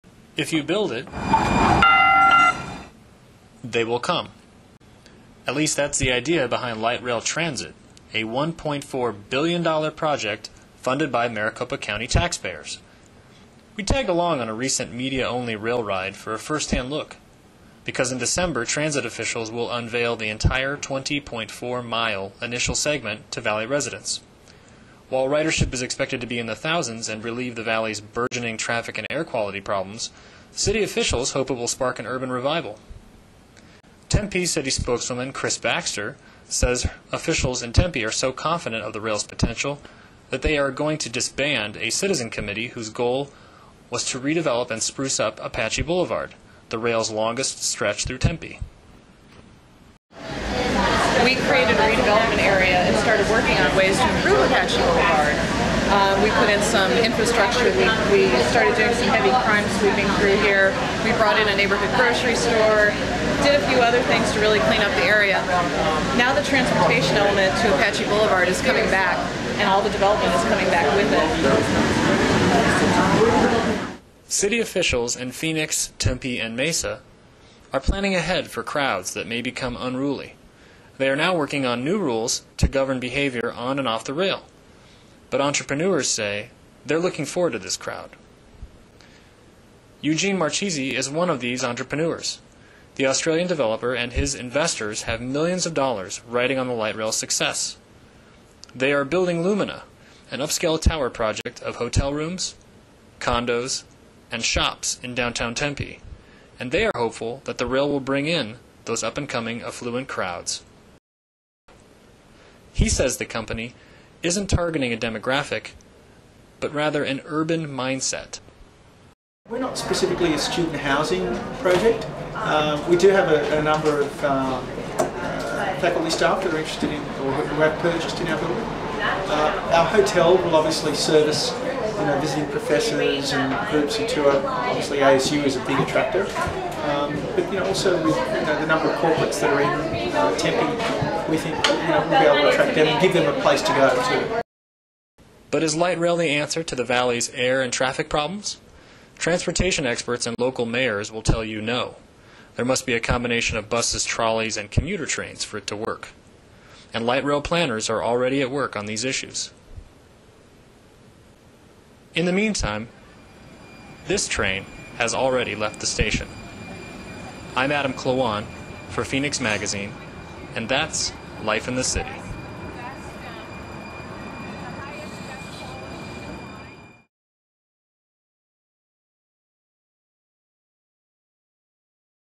Light Rail in Phoenix: A Sneak Peek
A first-hand look of the light rail in action. Two PHOENIX Magazine editors rode the rail for a media-only ride-along in October. Light rail officials will open the 20.4-mile track to the public on Dec. 27.